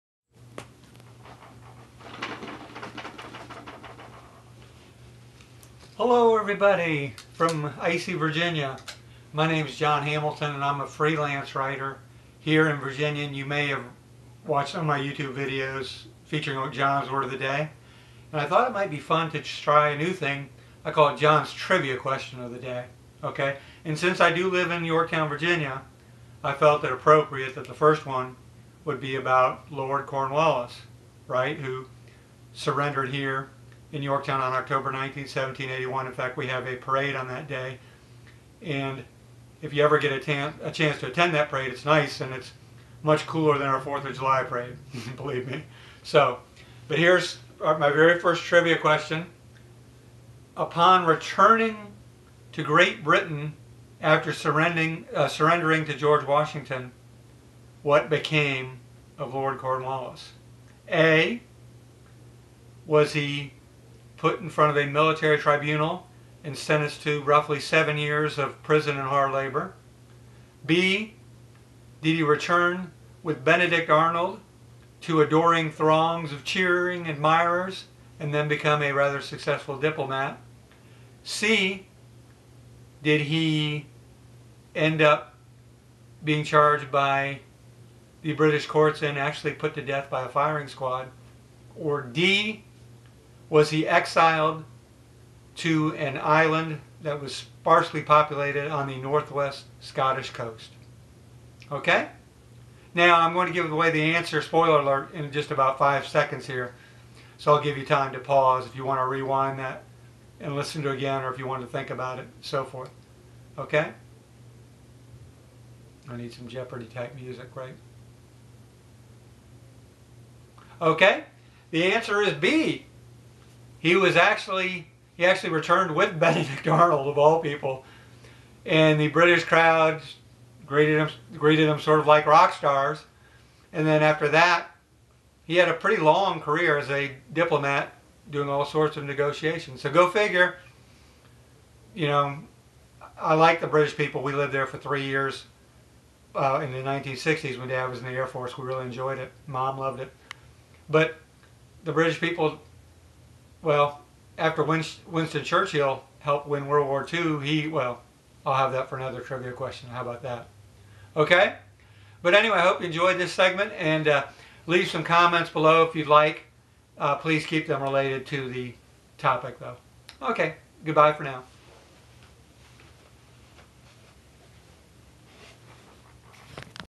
Urban Legends - What Became of Lord Cornwallis After He Surrendered at Yorktown?
Lord Cornwallis surrendered at Yorktown, Virginia on October 19th, 1781 (now known as Yorktown Day.) However, what became of him upon his return to England?